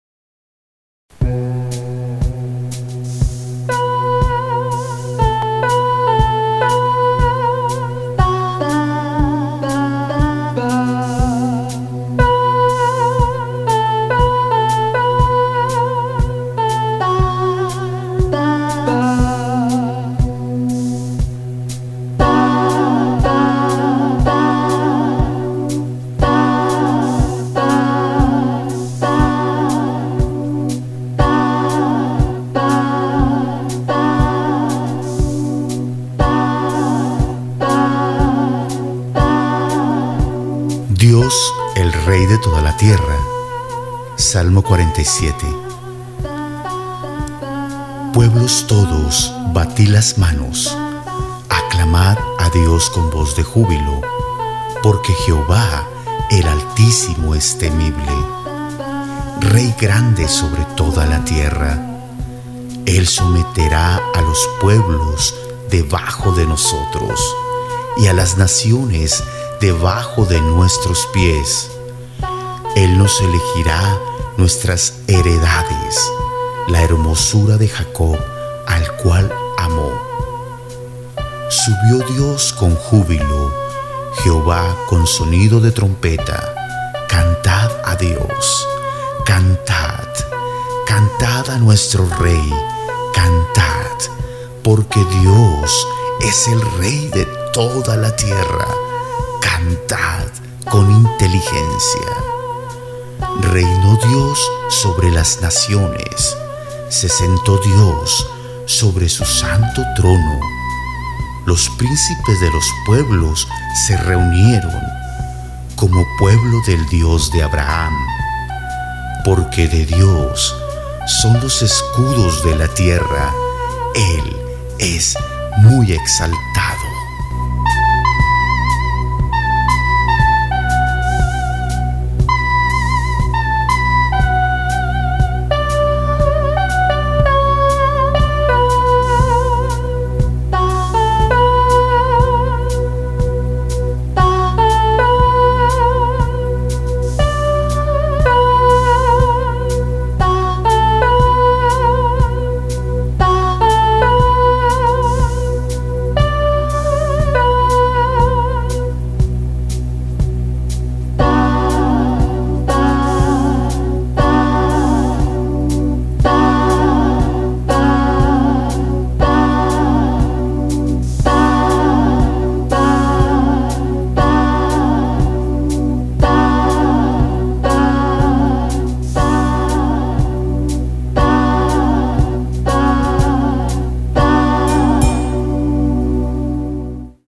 Salmo 47 Dios, el Rey de toda la tierra 7DíasTV
Bienvenidos al segmento "EL VERDADERO ALIMENTO PARA EL ALMA", de la cadena 7DíasTV "Una Puerta al Paraíso"; en esta oportunidad les estaremos presentando el SALMO 47 "DIOS EL REY DE TODA LA TIERRA"; esperamos sea de gran bendición para todos ustedes esta reflexión.